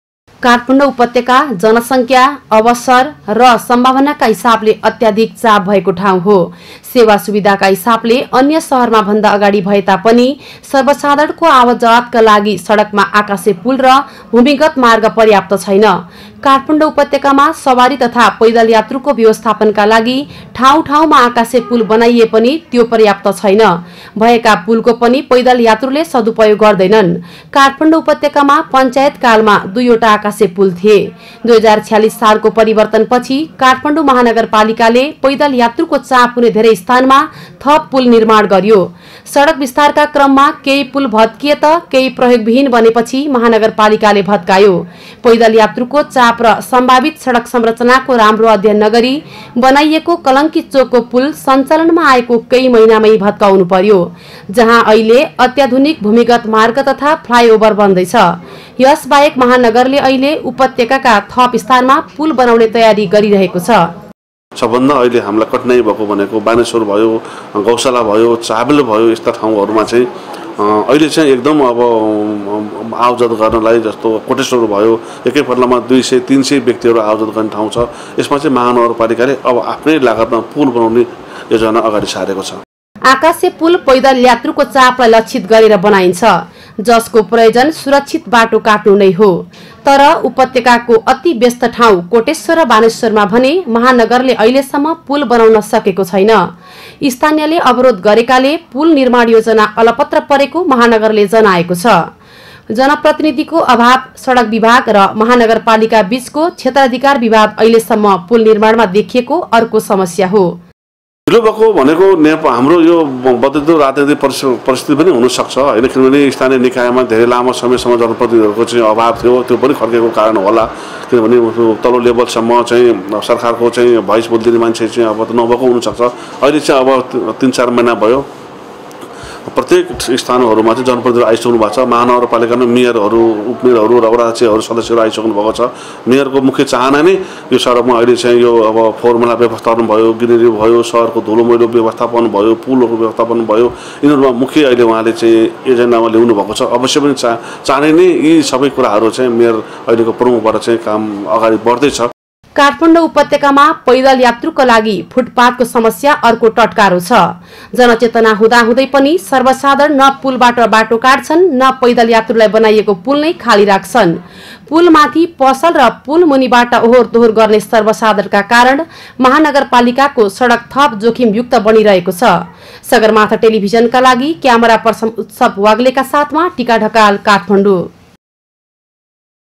काठमाडौमा आकाशेपुलको अभाव :
काठमाडौ उपत्यकामा दैनीक कति सवारी साधन गुड्छन्, कुनै तथ्यांक छैन । सांगुरो सडक तथा यो अस्तव्यस्त शहरमा कति सर्वसाधारण ओहोर दोहोर गर्छन् त्यो पनि यकीन छैन । तर उपत्यकाको चलायमान जनसंख्या ४० लाख भन्दा माथि छ र जनसंख्याको चाप अनुसार उपत्यकामा न्यूनतम पूर्वाधार नहुंदा पैदल यात्री पाईलै विच्छे, जोखीममा हुन्छन् ।